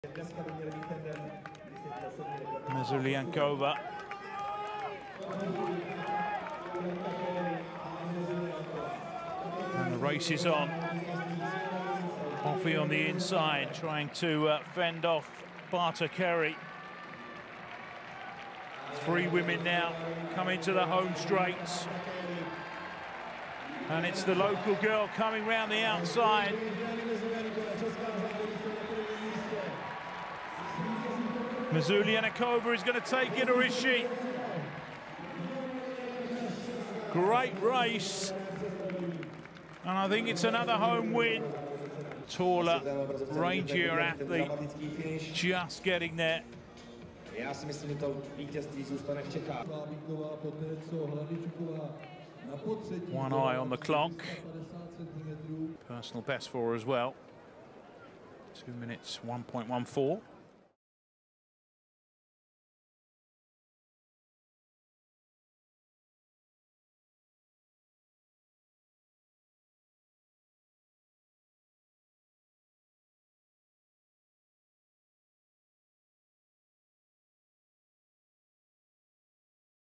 Photo Finish In Prague Continental Tour 800m!
Close finish in the women's 800m race at the 2022 World Athletics Continental Tour Prague.